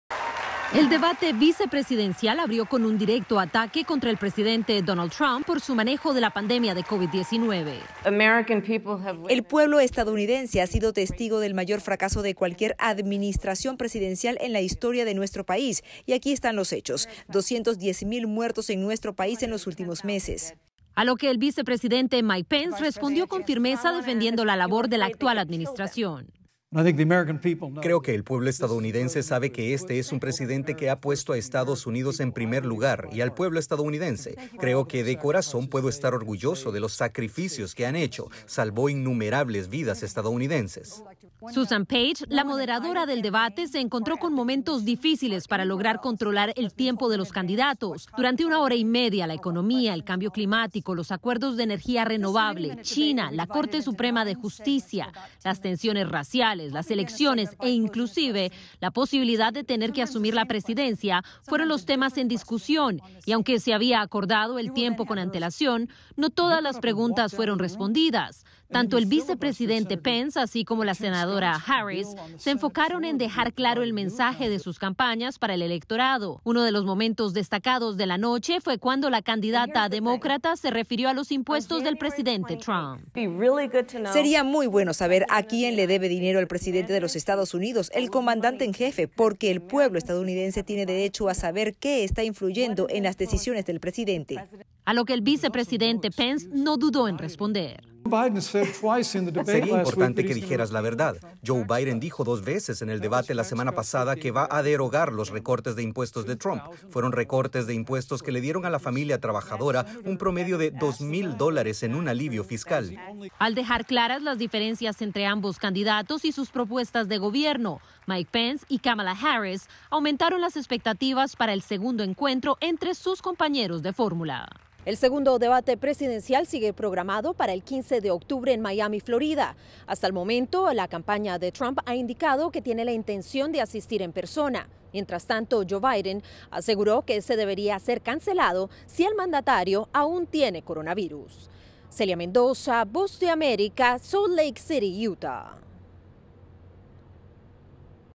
Harris y Pence finalizan debate reiterando mensajes de campaña y evitando contestar preguntas
En medio de grandes expectativas, se llevó acabo el debate de los candidatos a la vicepresidencia de Estados Unidos en la ciudad de Salt Lake City en Utah. Ambos aspirantes defendieron las políticas de sus campañas e intentaron desarmar el discurso de su oponente.

¡Visítenos!